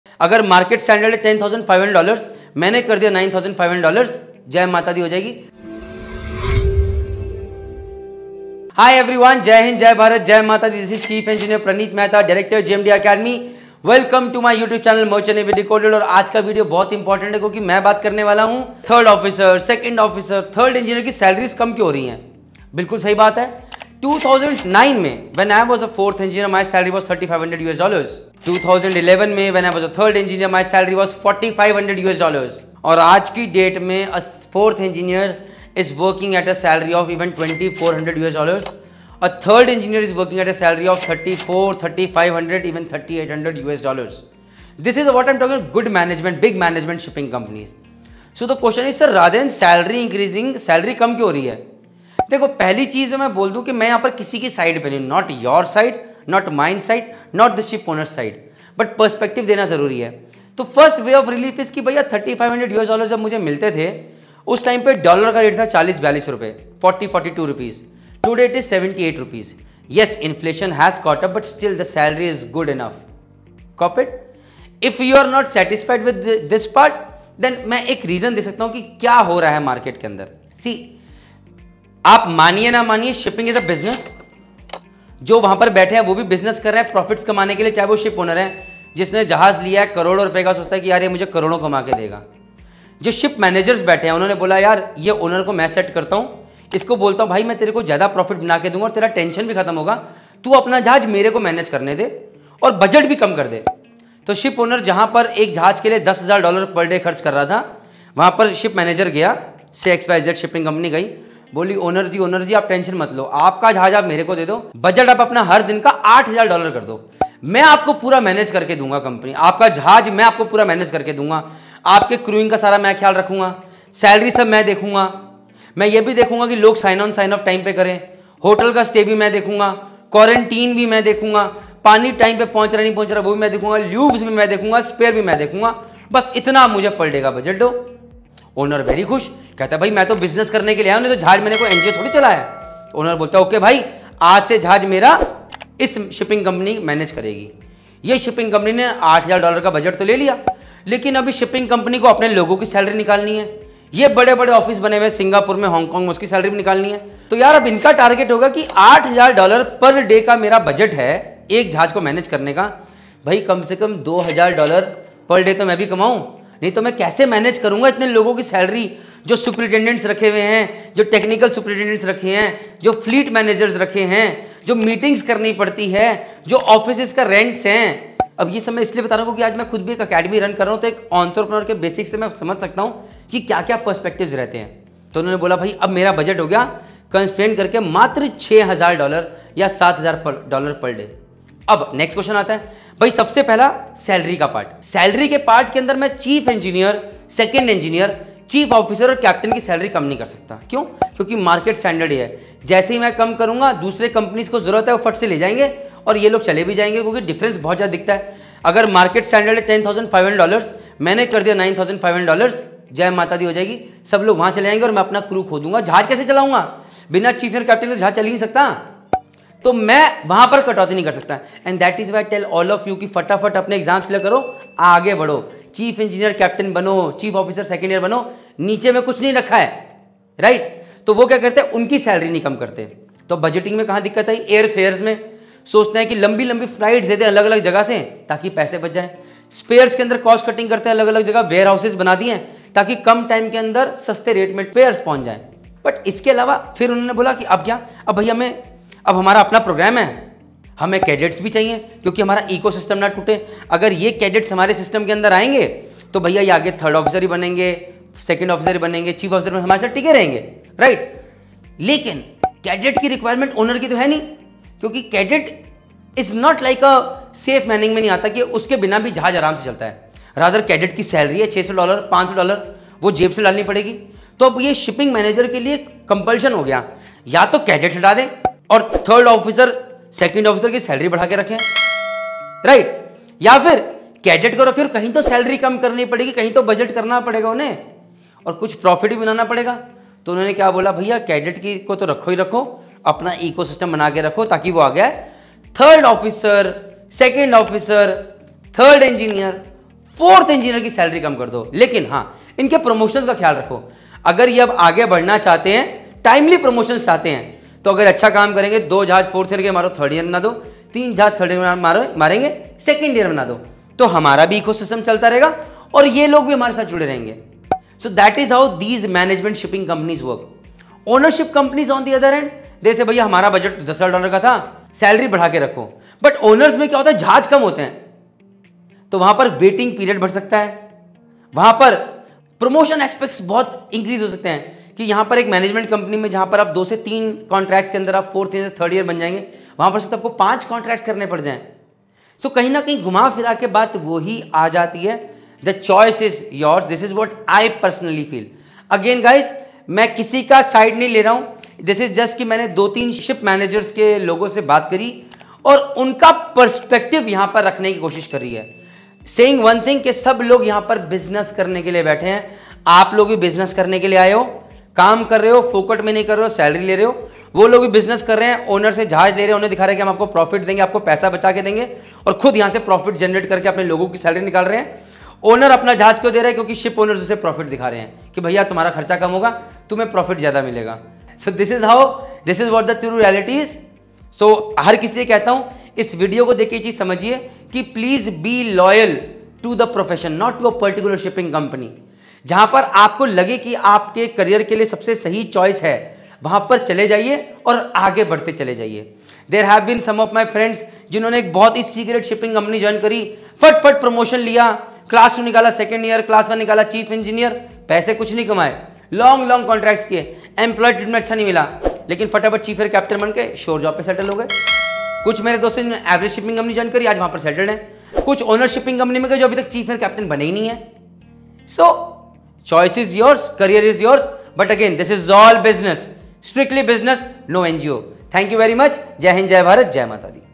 Salary Is Decreasing | Know The Real Truth
The following video is about the decreasing trend in salaries of junior officers in the Merchant Navy. Chief Engineer Praneet Sir explains the reason for this decreasing trend based on the present market situation in the shipping industry.

Enroll for free in our “ How to Join Merchant Navy series” to learn about Merchant Navy and the joining procedure.